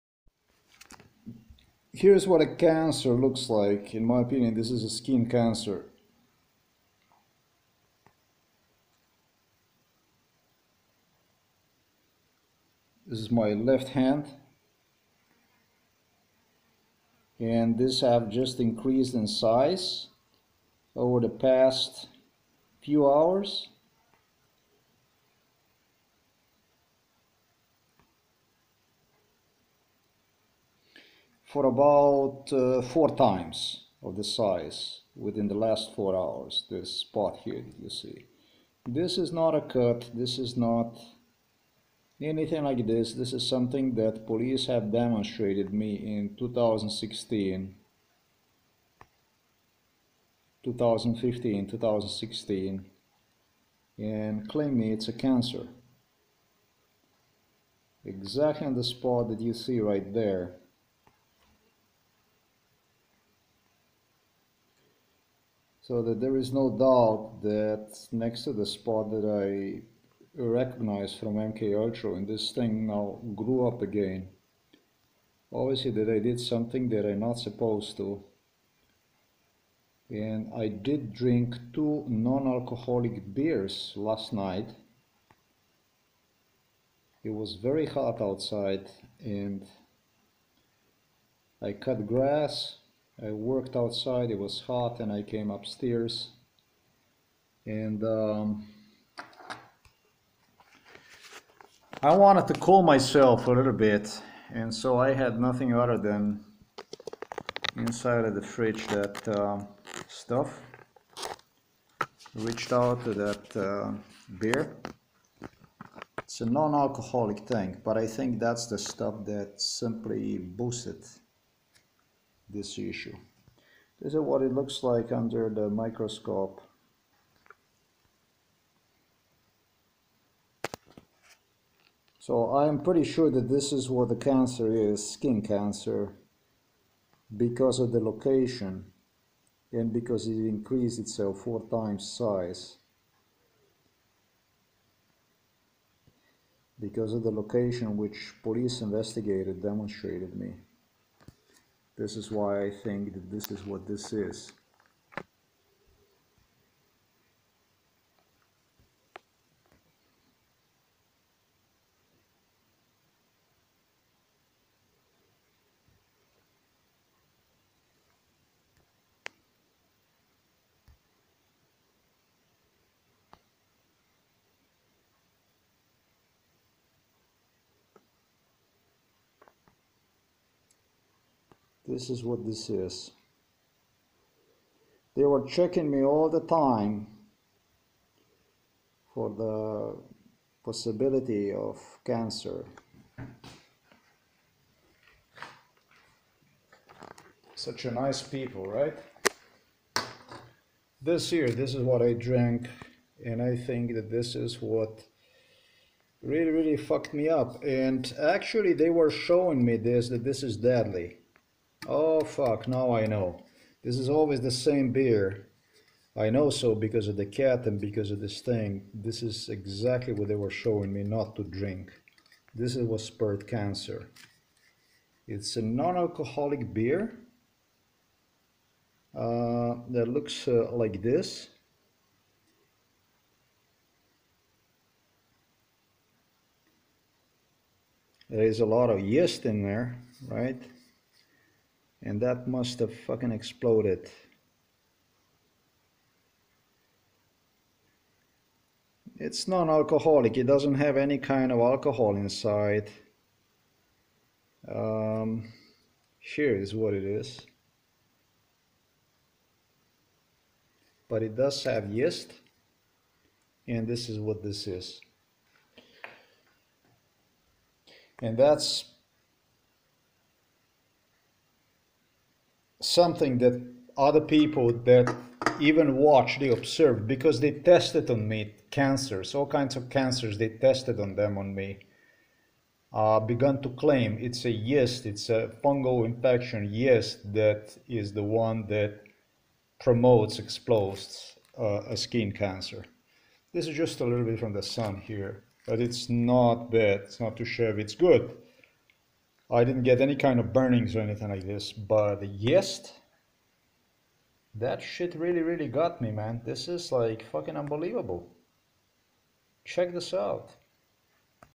1 of 2 Prince Charles KNEW NOTHING ABOUT SKIN CANCER QUIET AGREEMENT I made in private with police
1 of 2 Prince Charles KNEW NOTHING ABOUT SKINC CANCER QUIET AGREEMENT I made in private with police investigator IN 2015